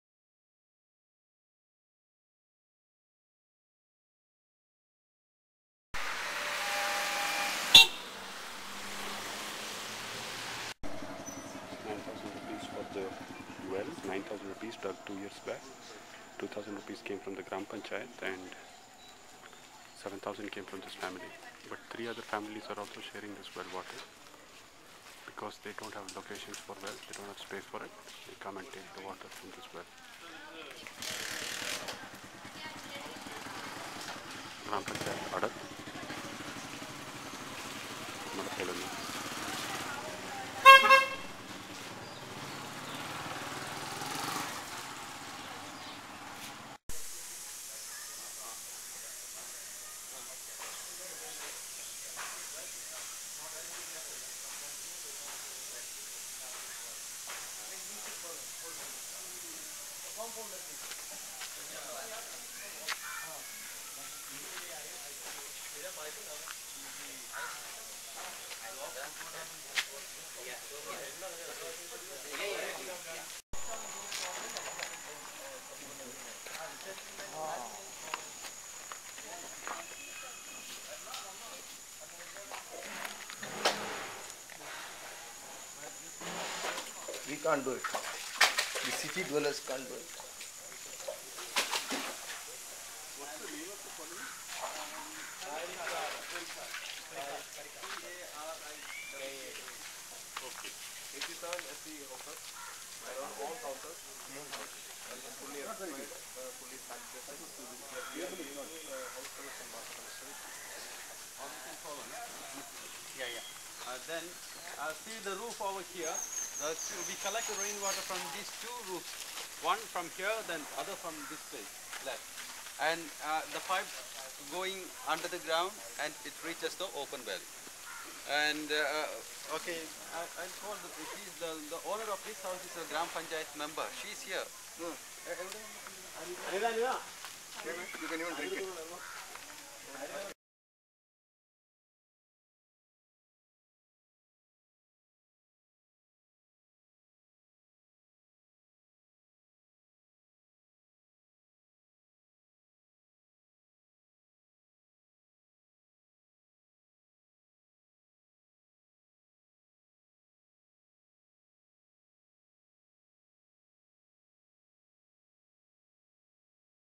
Mazhapolima - Rainwater harvesting to recharge open wells, Thrissur , Kerala
A brainchild of the District Collector Mr Kurian Baby, Mazhapolima is a project which seeks to create a people's movement on recharging open wells through rainwater harvesting. This effort is being supported by Arghyam for the documentation and the monitoring and evaluation part.